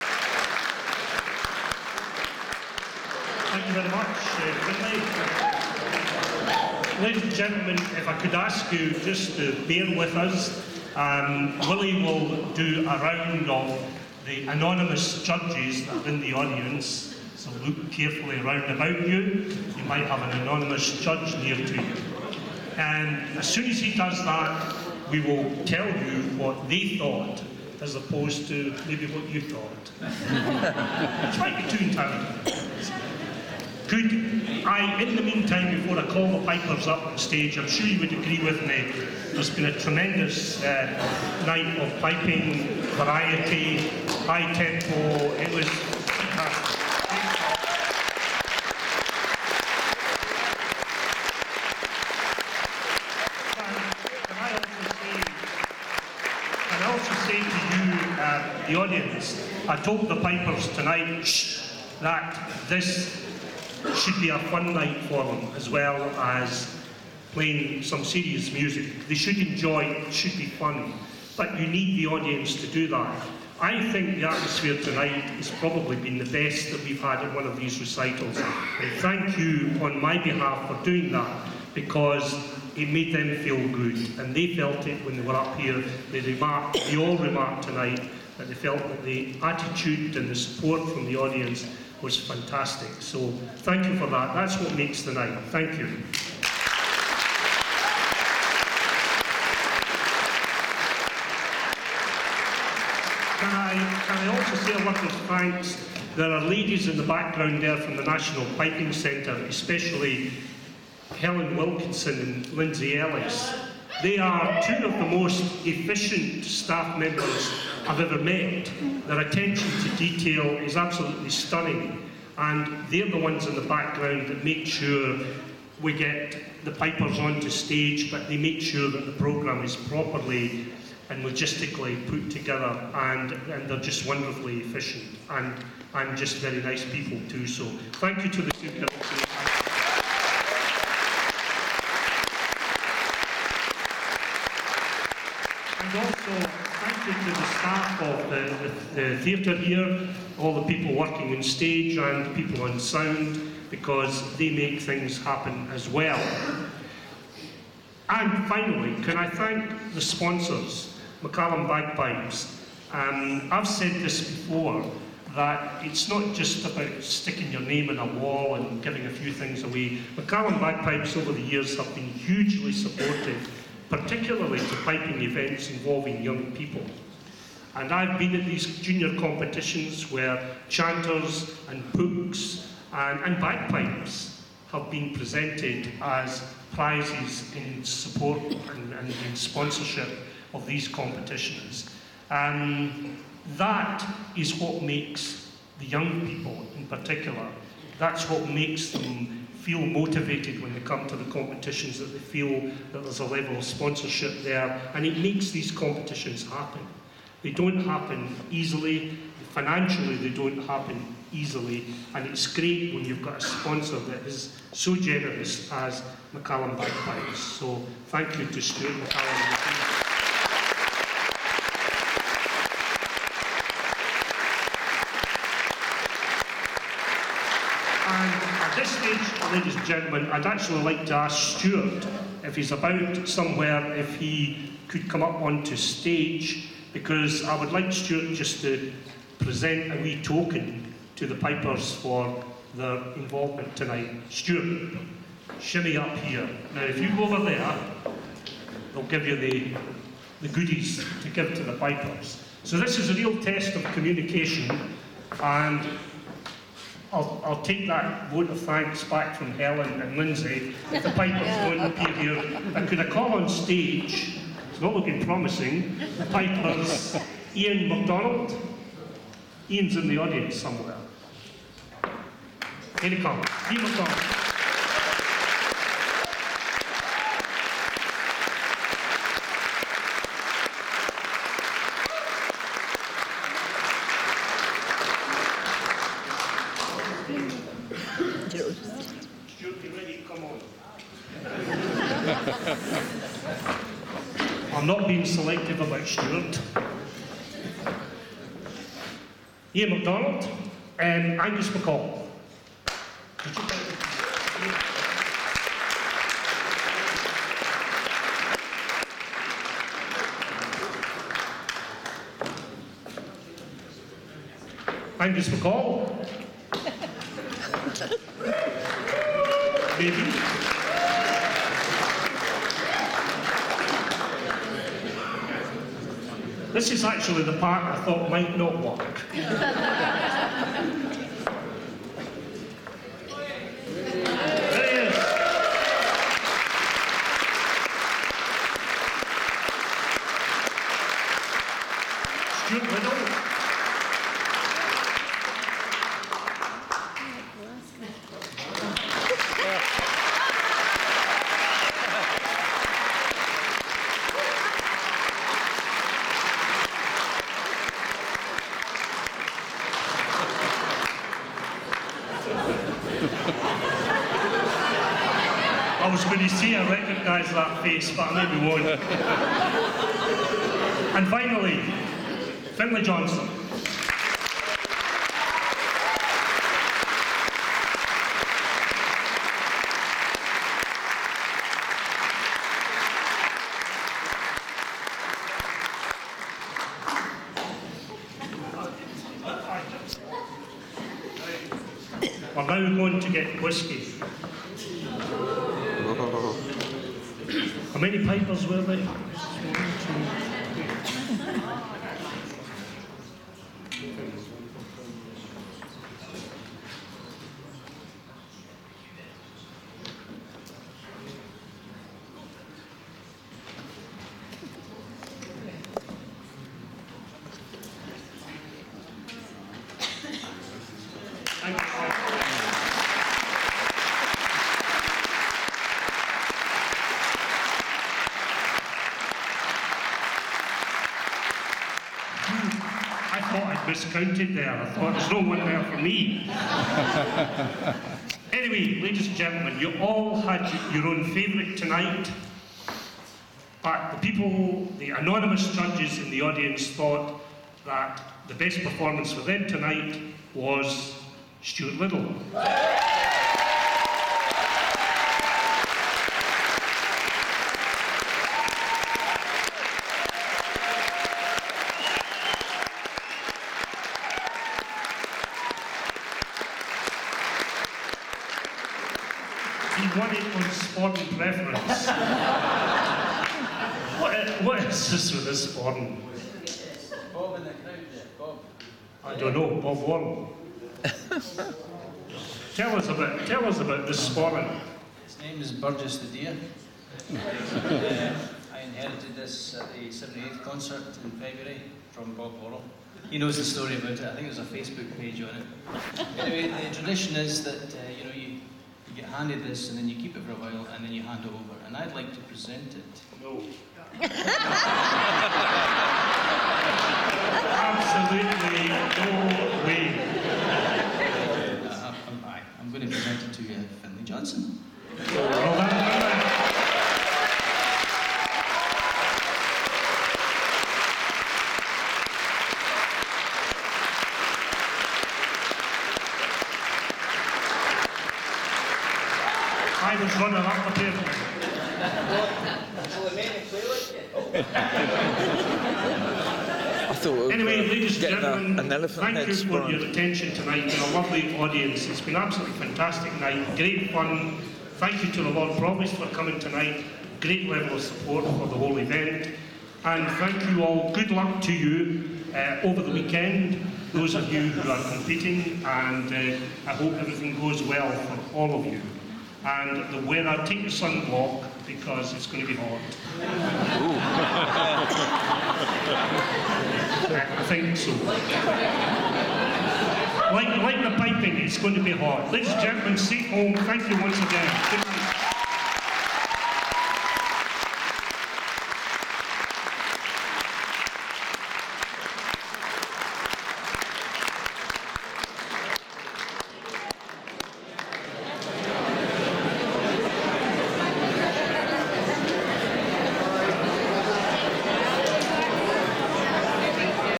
P/M Gillies Memorial 2017 - THE RESULTS
John Wilson gives the results and Stuart McCallum (of McCallum Bagpipes) hands out the prizes.  Stuart  Liddell wears his Deer's Head Sporran  - Burgess the Deer, and presents it to Finlay Johnstone !